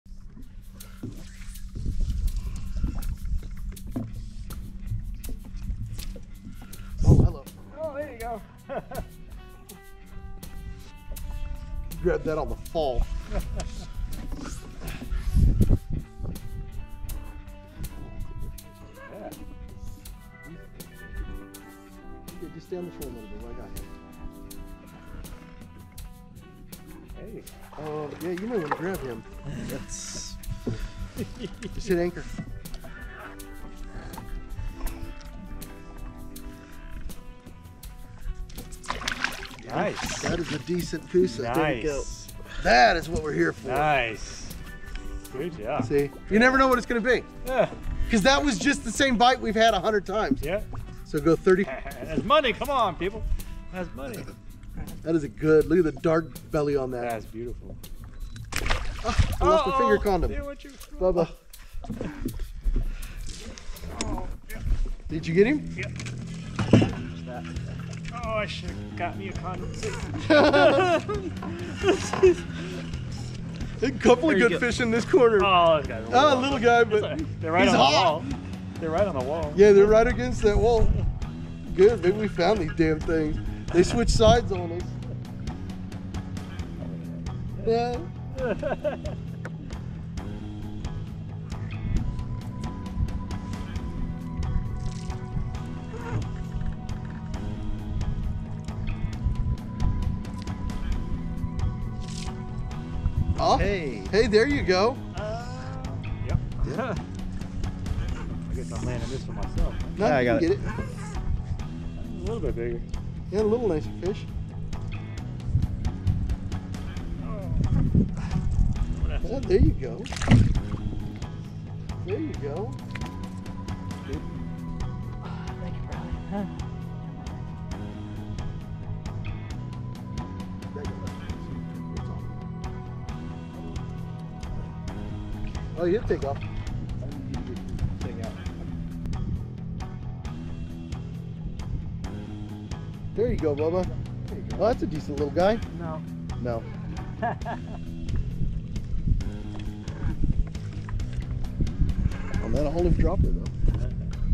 Morning of Laughs With the float and fly at Lake Tulloch California
Morning of laughs with the float and fly at Lake Tulloch California. Our buddy Jeff laughed all morning at how aggressive these little smallmouth and red eyes are in the lake￼. This was his first time out with us I was really excited to fine-tune his float and fly skills. I think he’s got it figured out after a day of catching and missing.